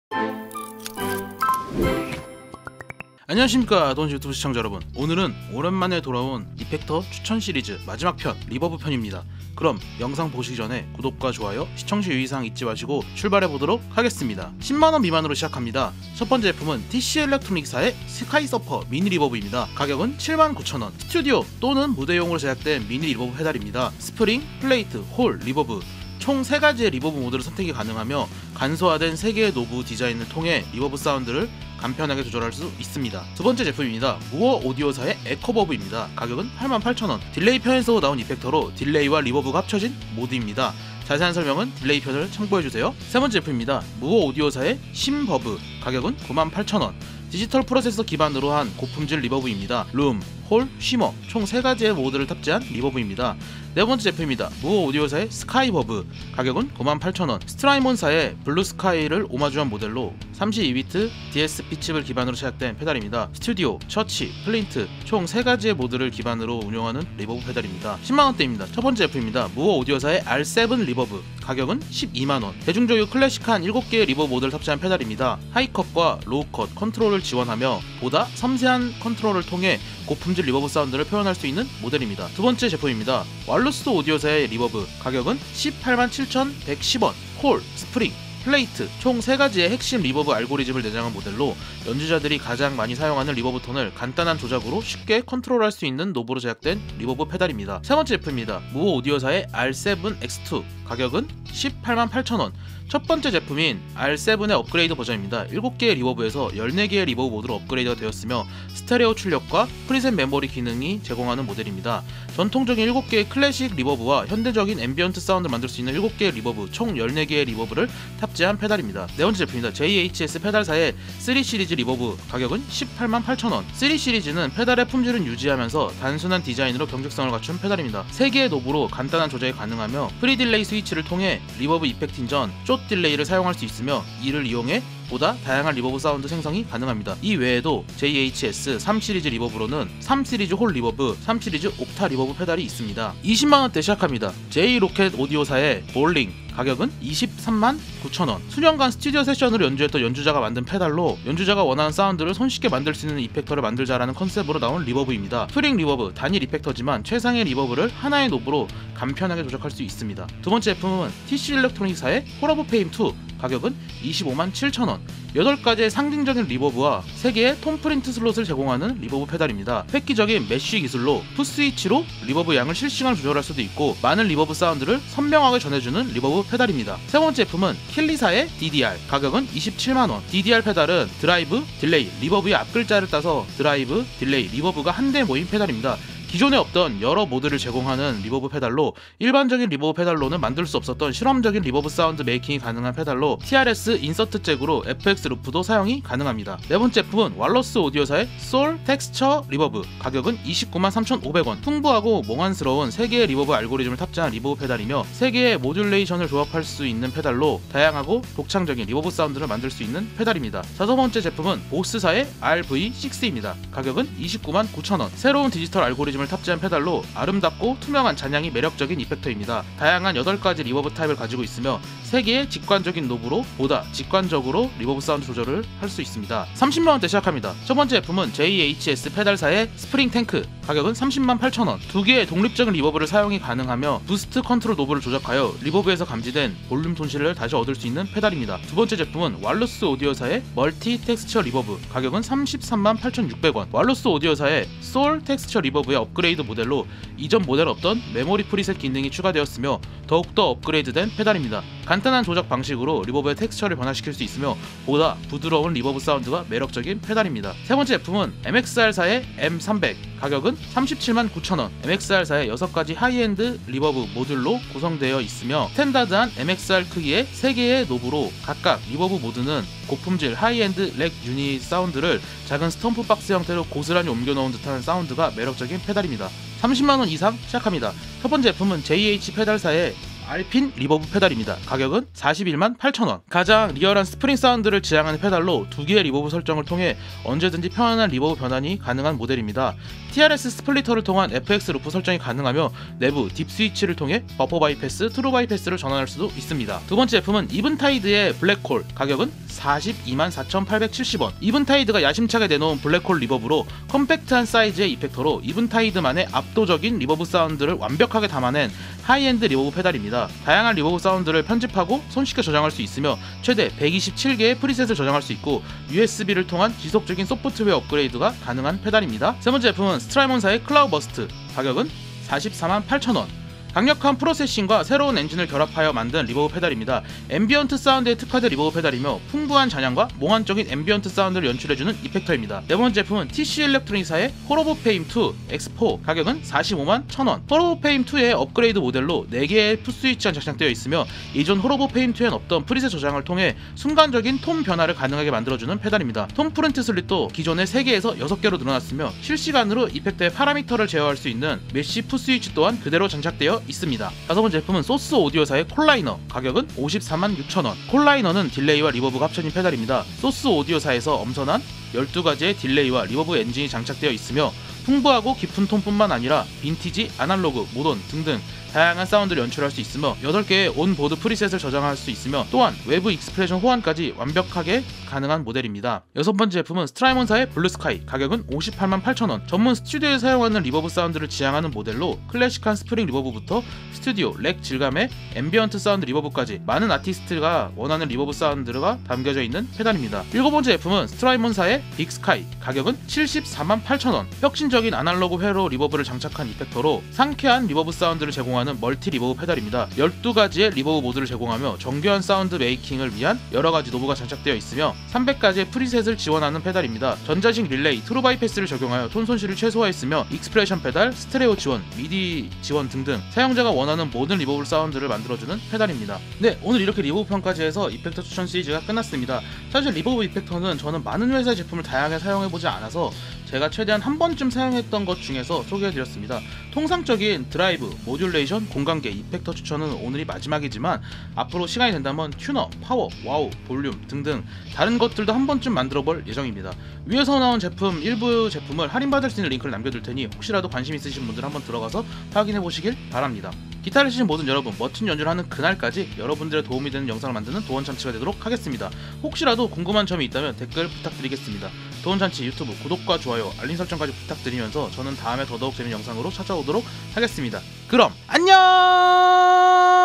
2023년 가격대별 이펙터 추천영상(리버브 편) 공간계의 꽃 리버브입니다
가격대별 이펙터 추천영상 리버브 편입니다

▶영상에 나온 리버브 일부 구매링크

0:00 인트로
0:14 10만원 미만
1:08 10만원대
2:22 20만원대
3:46 30만원대
4:36 40만원 이상
6:55 이펙터 추천 시리즈를 마치며
7:39 할인 링크
7:24 아웃트로